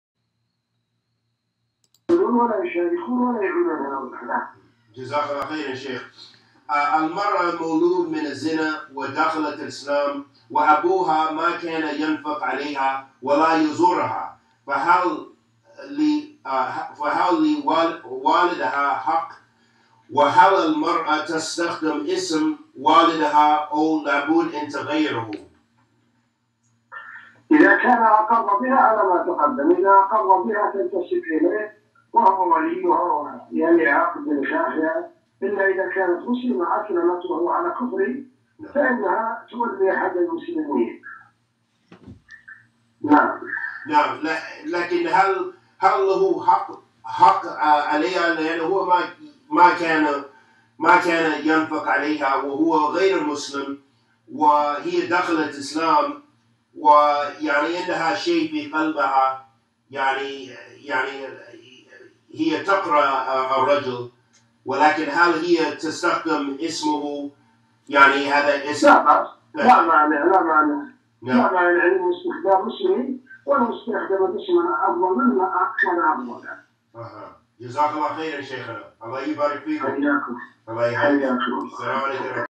My Father Abondoned Me Before Islam| Shaikh Saeed Bin Halayl
الشيخ سعيد بن هليل العمرIslam and the Sunnah is simple so follow it and avoid sects